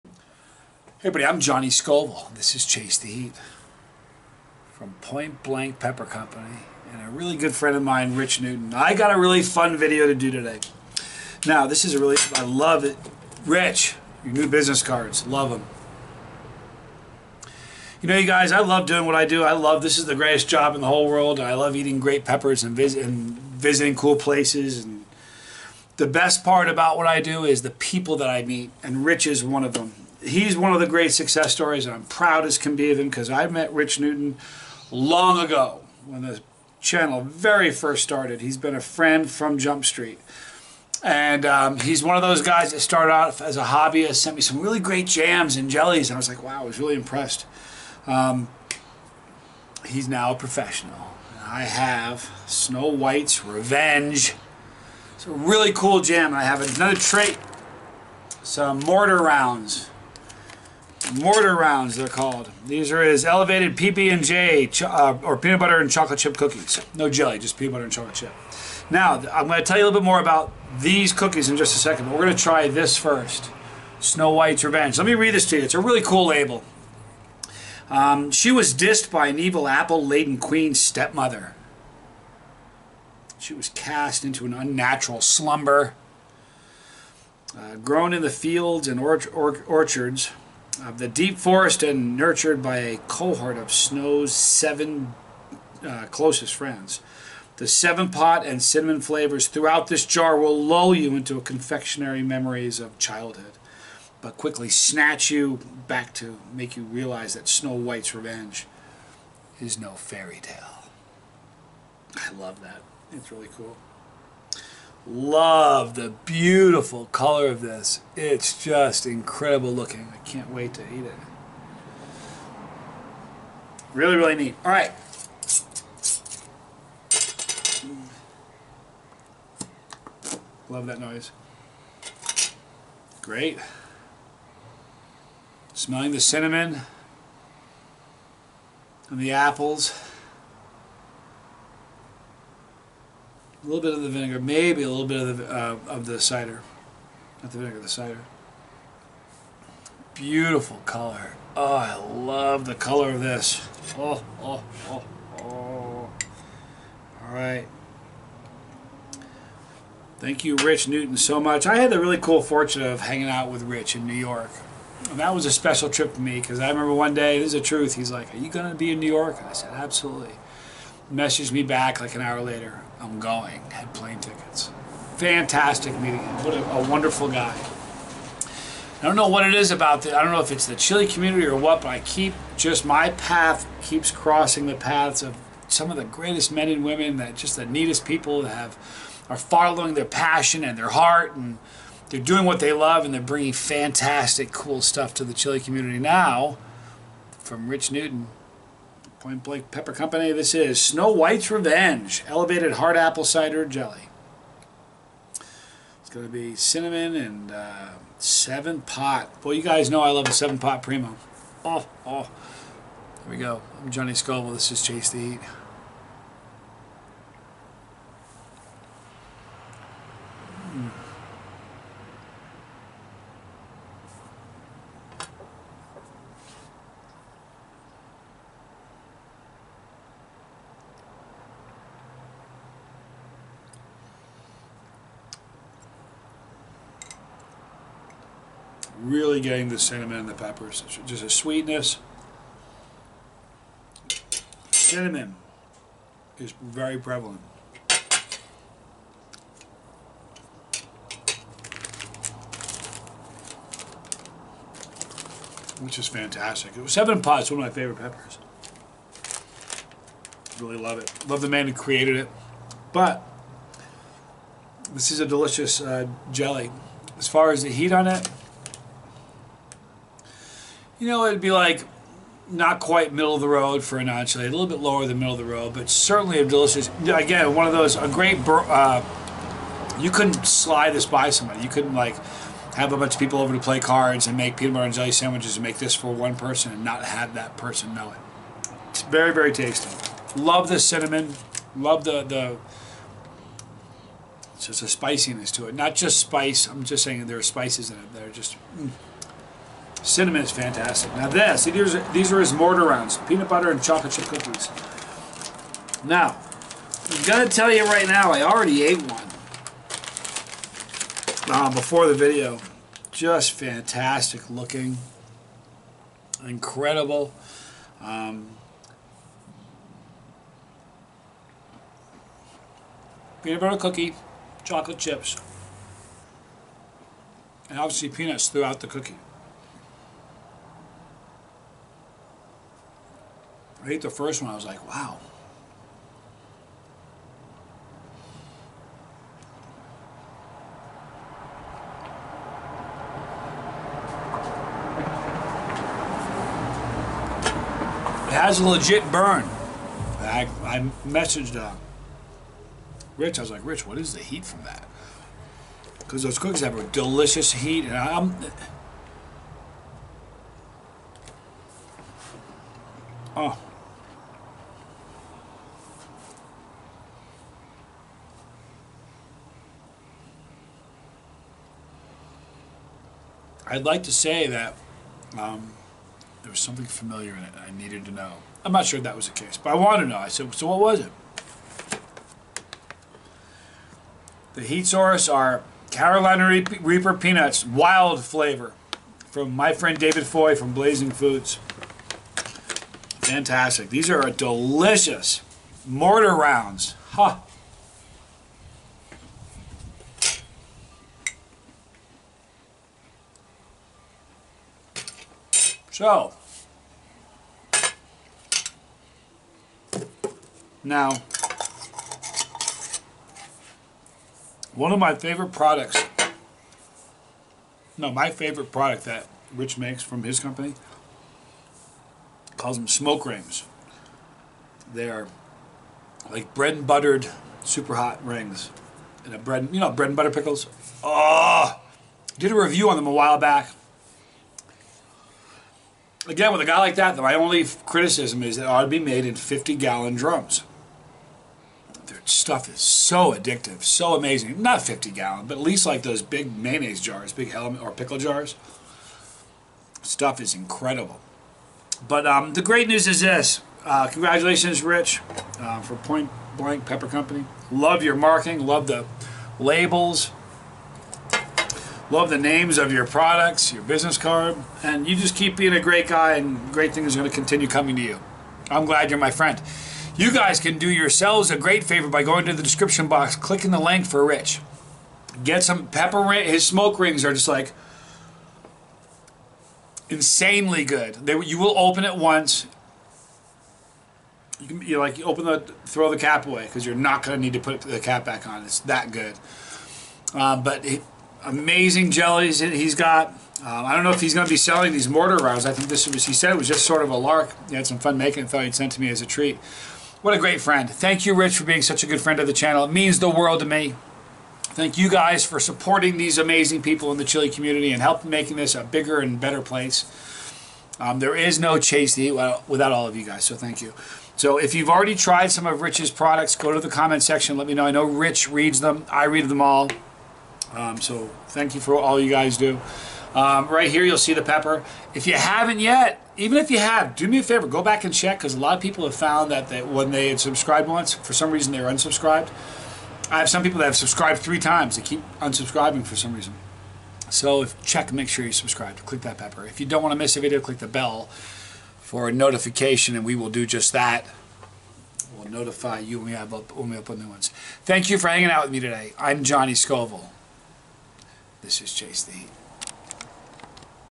SNOW WHITE'S REVENGE AND MORTAR ROUNDS FROM POINT BLANK PEPPER CO. AND MY FRIEND RICH NEWTON!!
Get your Chase the Heat Charter Member Shirt!

Rich makes some amazing products!! You need to click the link below and get some and let me know what you think!!

CHASE THE HEAT!!!

Johnny Scoville